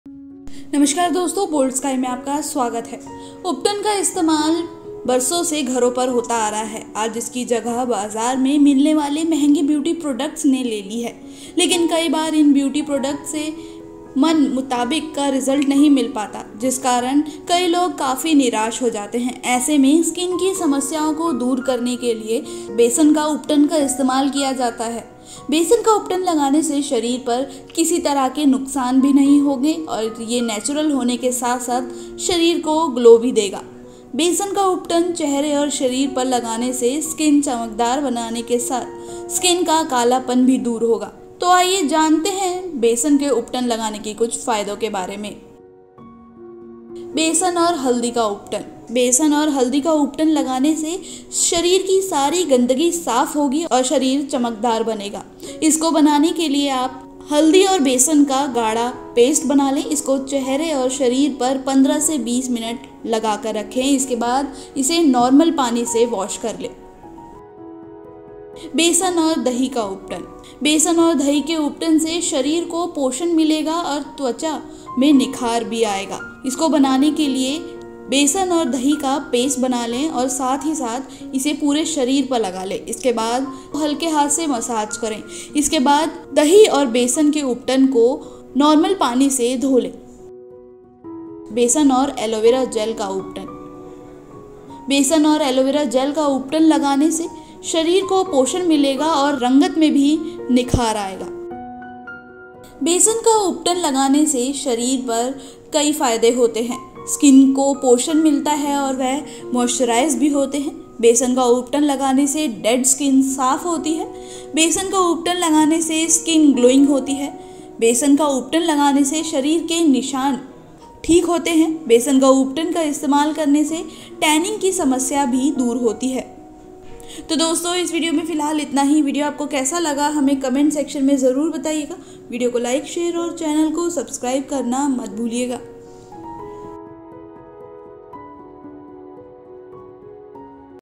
Body पर Besan का Ubtan लगाने से क्या होता है । Body पर Besan का Ubtan लगाने के फायदे।Boldsky*Health
उबटन का इस्तेमाल बरसों से घरों पर होता आ रहा हैं। आज इसकी जगह बाजार में मिलने वाले महंगे ब्यूटी प्रोडक्ट्स ने ले ली हैं। लेकिन कई बार इन ब्यूटी प्रोडक्ट्स से मन मुताबि क रिजल्ट नहीं मिल पाता। बेसन का उबटन लगाने से शरीर पर किसी तरह का नुकसान भी नहीं होगा ।

Ubtan has been used at homes for years. Today it has been replaced by expensive beauty products available in the market. But many times these beauty products do not give desired results. There will be no harm on the body by applying gram flour.